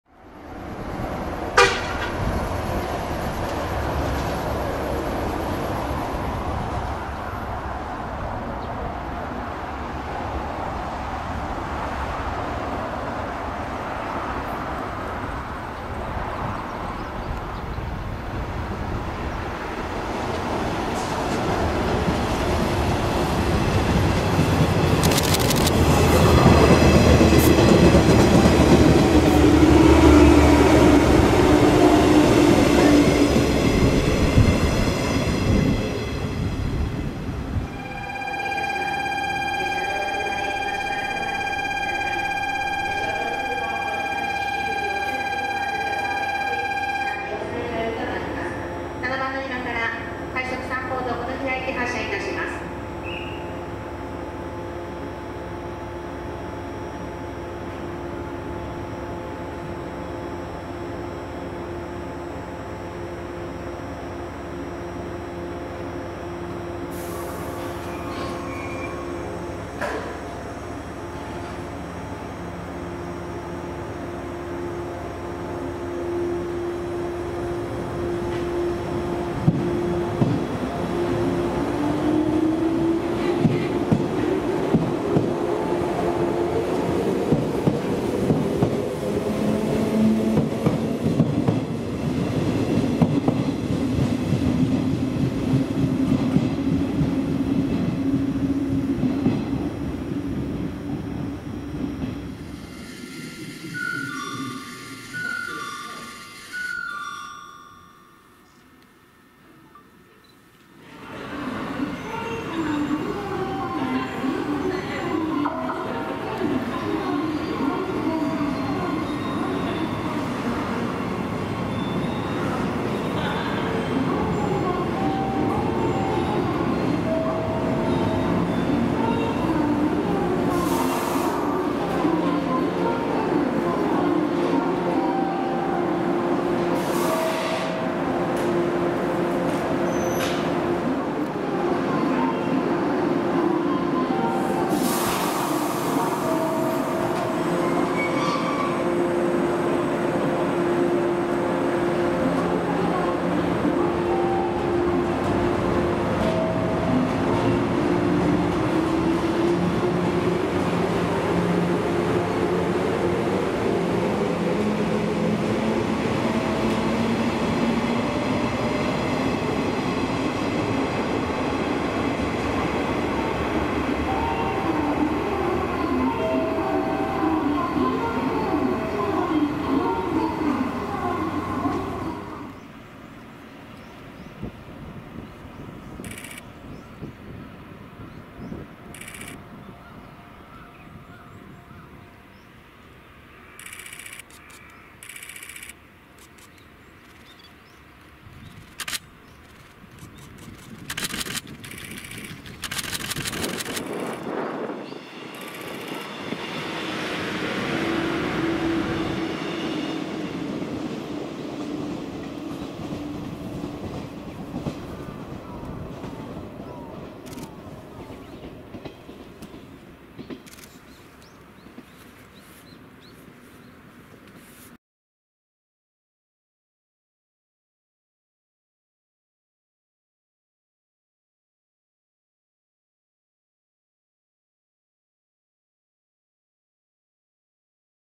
JR四国113系走行動画集
2019年3月のダイヤ改正をもってJR四国の113系が引退することとなりました。
中々四国に行く機会がなく十分な記録が出来ませんでしたが手持ちの動画をまとめました。
もとはJR東日本の車両でJR四国が購入し改造を施して現在の姿になっています。
元の姿が想像が付かないほど改造されカラフルで面白い車両でした。